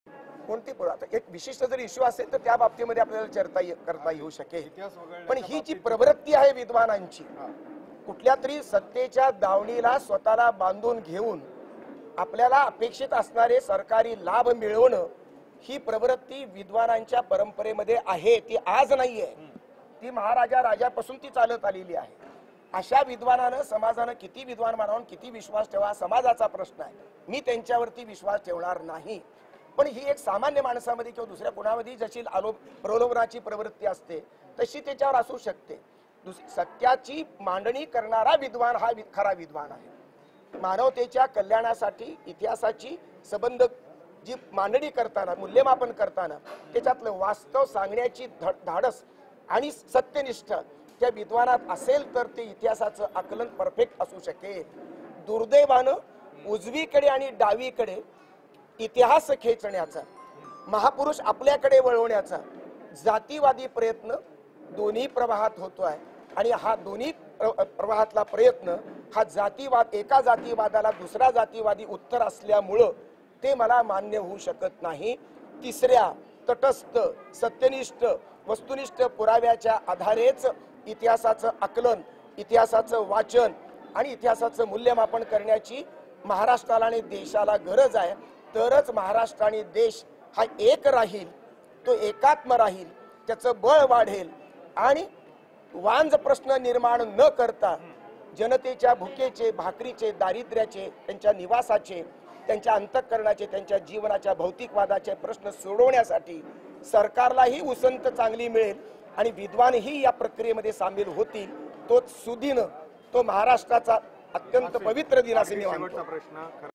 तोपर्यंत असले वांझोटे वाद सुरूच राहतील - डॉ. श्रीपाल सबनीस
पिंपरी - गणेशोत्सवाचे आद्य संस्थापक लोकमान्य टिळक की भाऊ रंगारी या सध्या सुरू असलेला वाद मुळात वांझोटा आहे. रंगारींनी घरातला गणपती, सार्वजनिक गणपती म्हणून रस्त्यावर आणला असेल तर त्या उत्सवाचे पहिले श्रेय त्यांना द्यायलाच हवे. महापुरूषांमध्ये जसे सामर्थ्य असते, तसेच मर्यादाही असतात, याचं भान समाजात जोपर्यंत येत नाही, तोपर्यंत असले वांझोटे वाद सुरूच राहतील, असे डॉ. श्रीपाल सबनीस यांनी सांगितले.